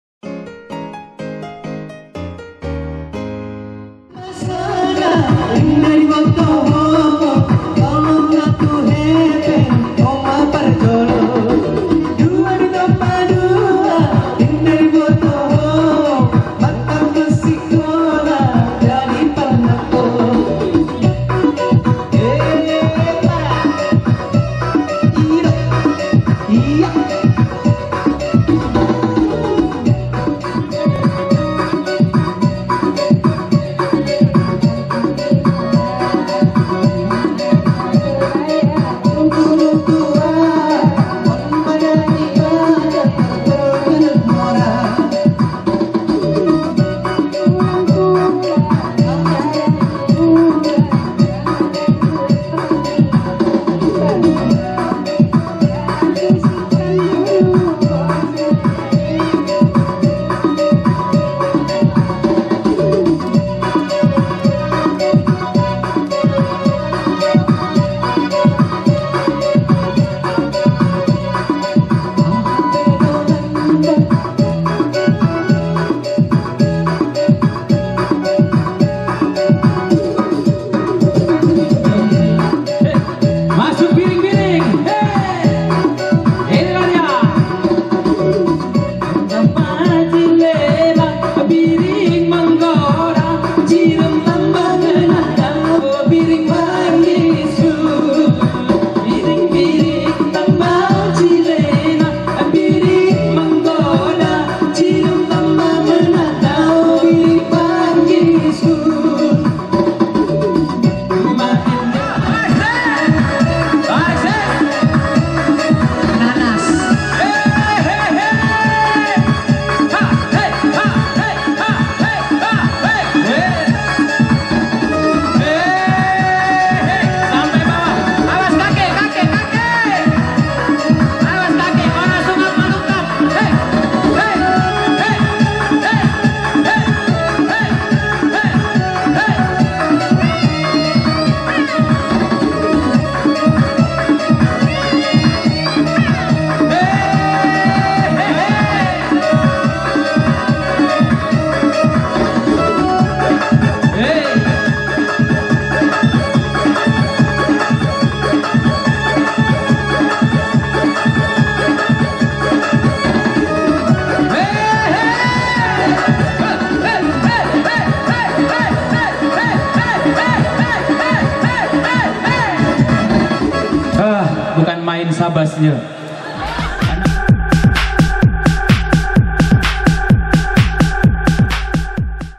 Endeng Endeng Kibot Mendayu Dayu
musik kibot endeng endeng asal mandailing memang luar biasa